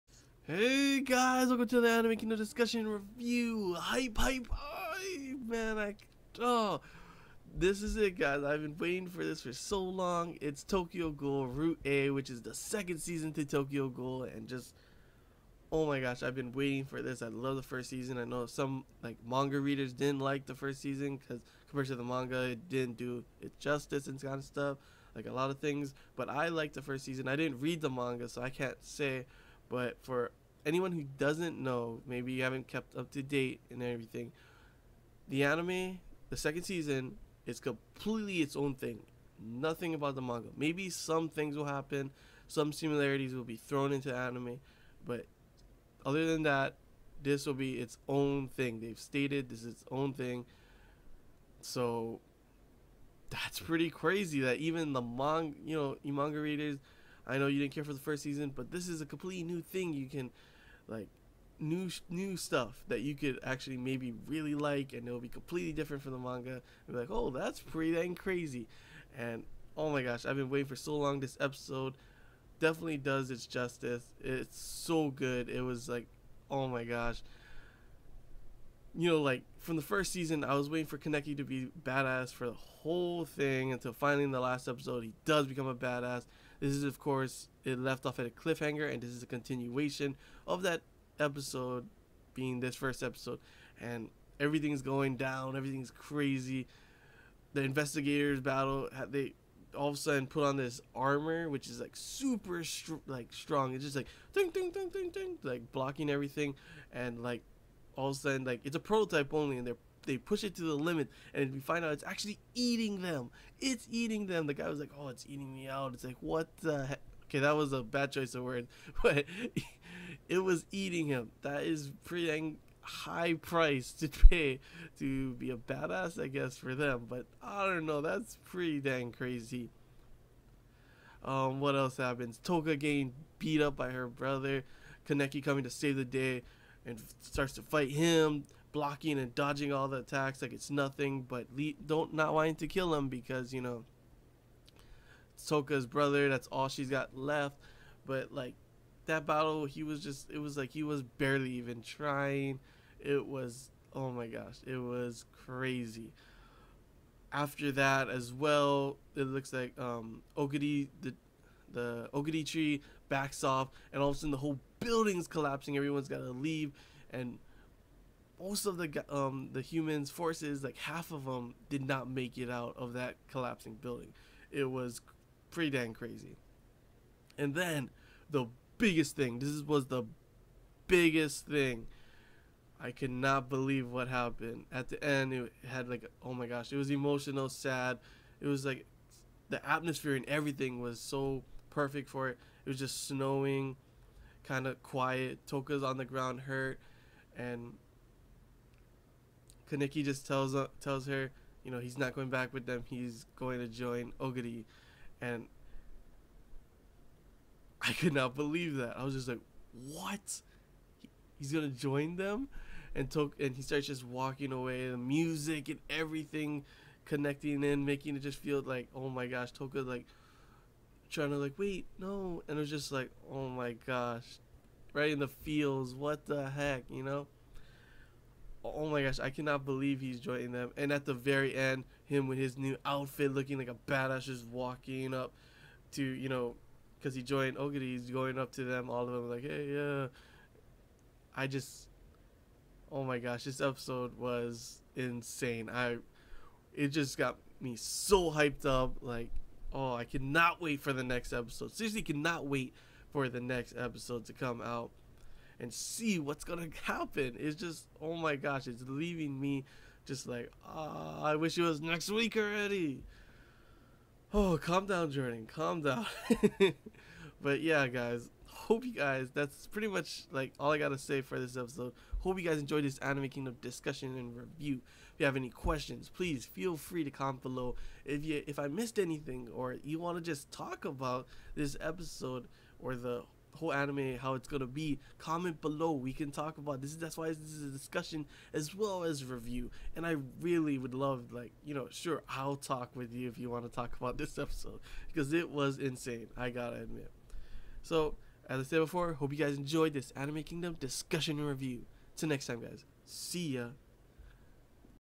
Tokyo Ghoul √A Episode 1 Discussion and Review
Hey everyone! SECOND SEASON OF TOKYO GHOUL IS OUT!!! HYPE HYPE HYPE!!! I discuss what I saw and what I liked in the first episode of Tokyo Ghoul √A.